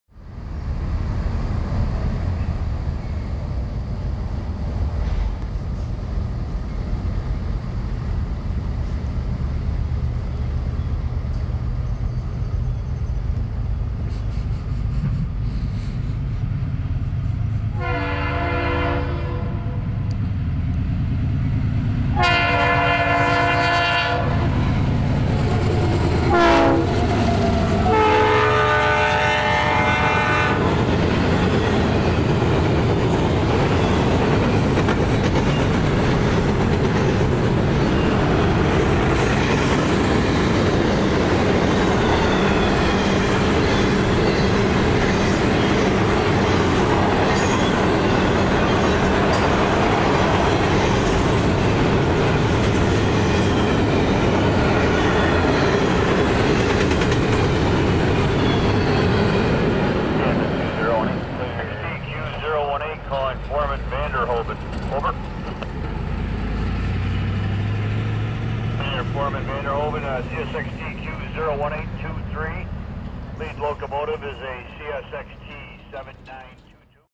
(5) CSX Q018 @ Westfield Sept. 24 2015
An east bound CSX intermodal train, Q018 comes past Westfield, New York. September 24 2015.